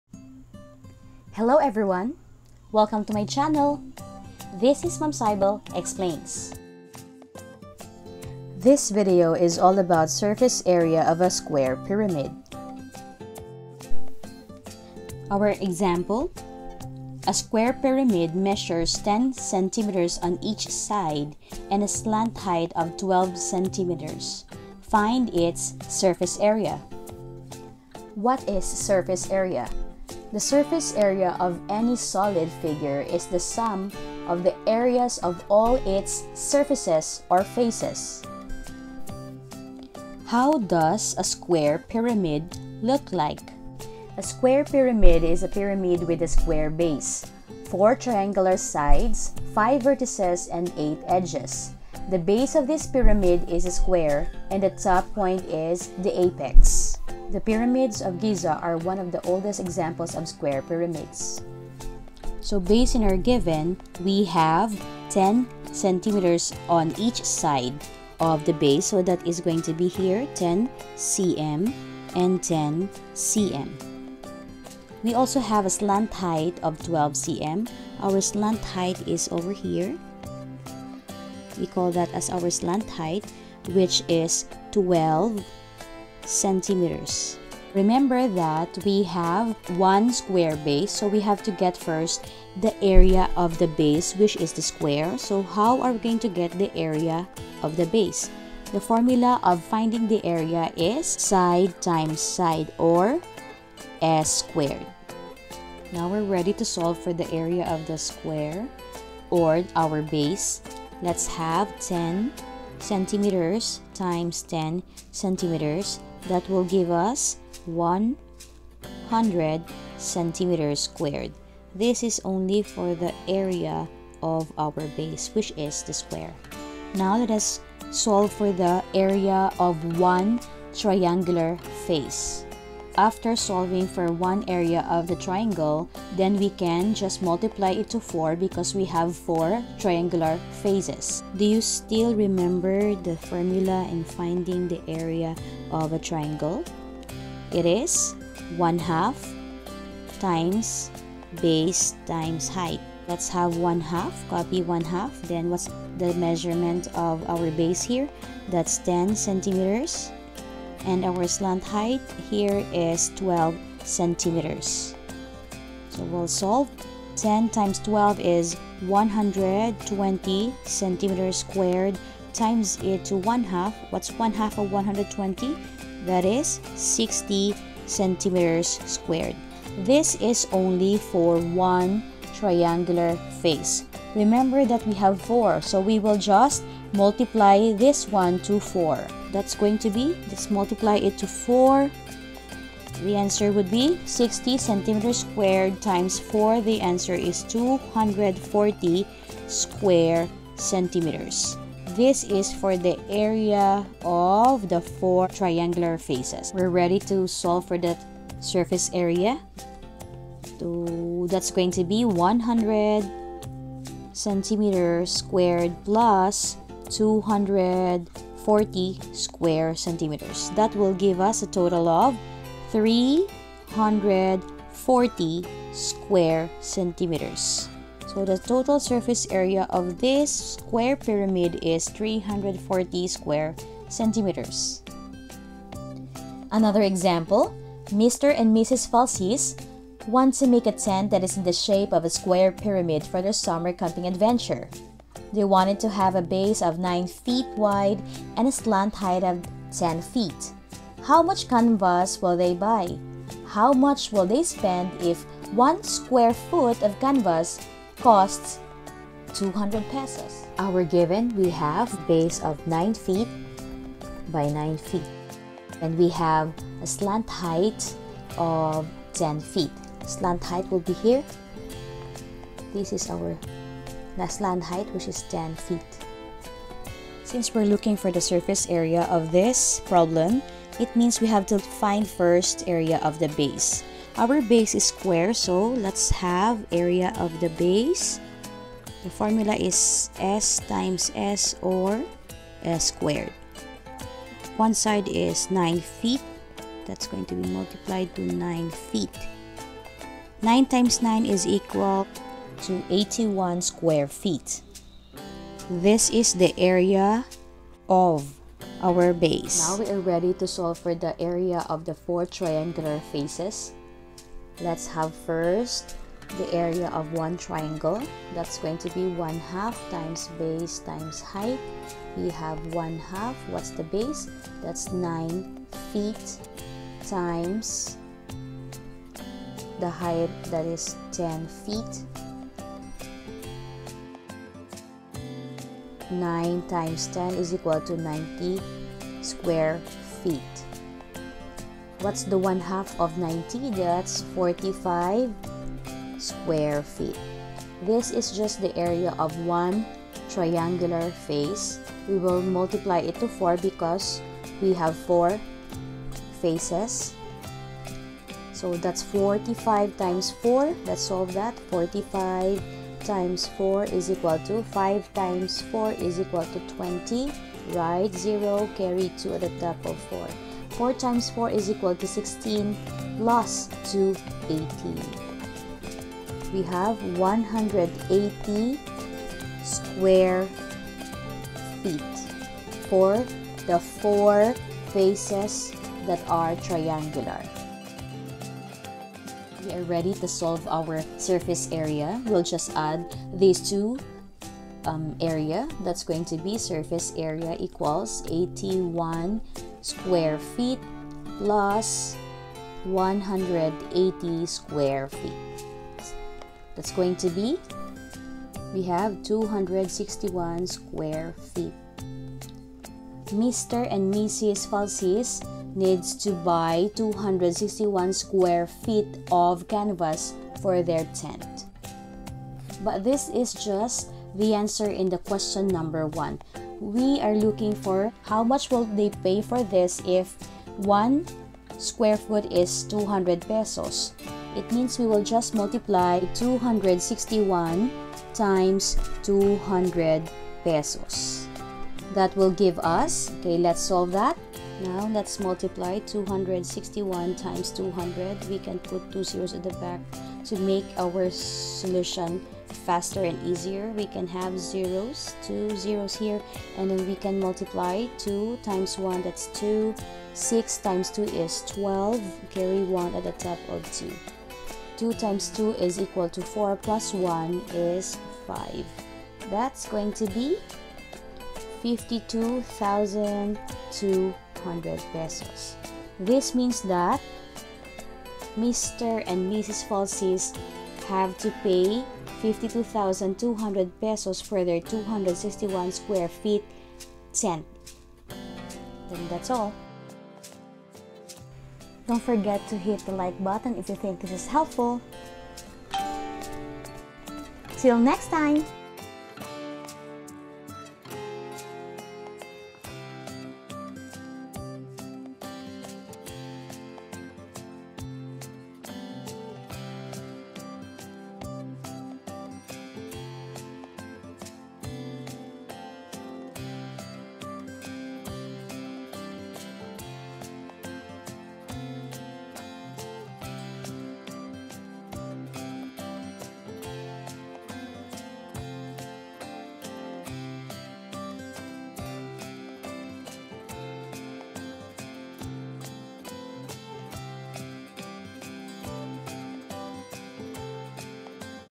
Surface Area of a Square Pyramid (Word Problems)
This video is all about the surface area of a square pyramid. A detailed discussion of the lesson was presented.

Happy Learning!

Timecodes
0:00 - Intro
0:14 - 1st word problem
3:56 - 2nd word problem

References:
Apistar, et. al.,  (2011) 2nd Edition Soaring 21st Century Mathematics 6. Phoenix Publishing House. Inc., Quezon City
Kotah, et. al.,  (2011) 2nd Edition Soaring 21st Century Mathematics 5. Phoenix Publishing House. Inc., Quezon City